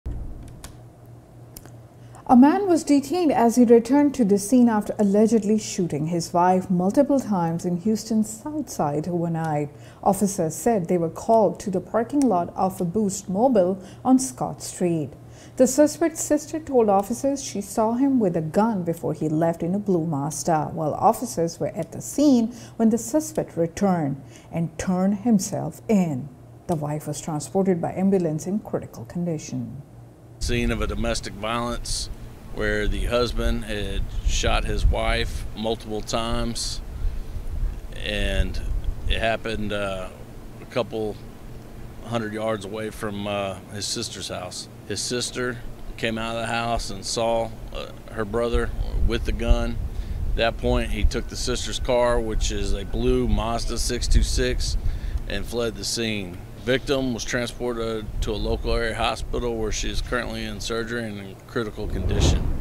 Man accused of shooting wife multiple times in southeast Houston before turning himself in
NTV America is available on television as well as Apple TV, Roku, Amazon Fire TV, Android Play, iStore.